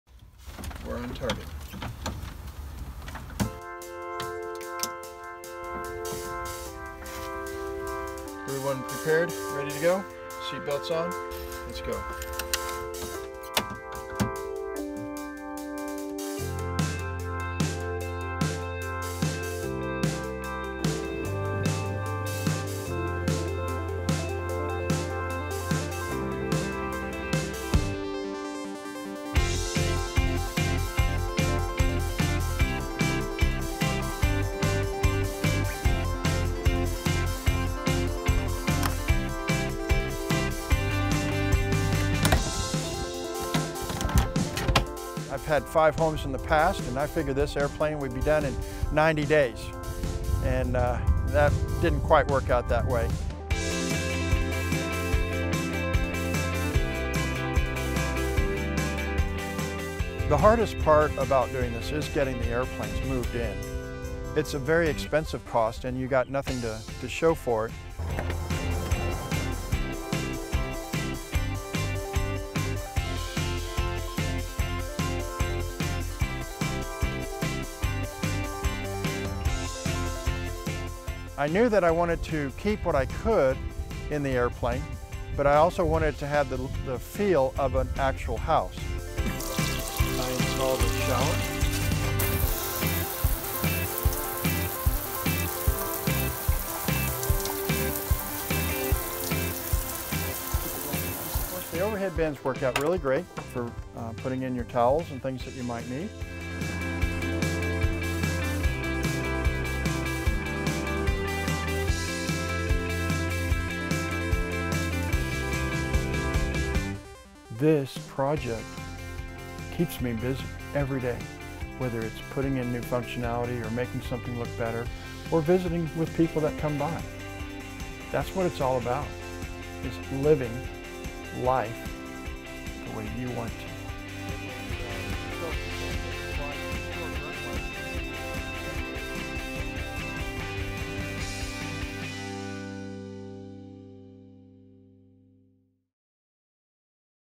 See how this guy made a home out of an airplane
Joe Axline’s love affair with airplanes began at an early age. He grew up outside Dallas, and his childhood dream was to convert a jetliner into a home. After a divorce in 2011, he purchased property in a community of runway homes west of Houston. Then he located two commercial aircraft that were going to be scrapped. CGTN's Mike Miller reports.

Follow CGTN America:
Google+: CctvamericaTvnews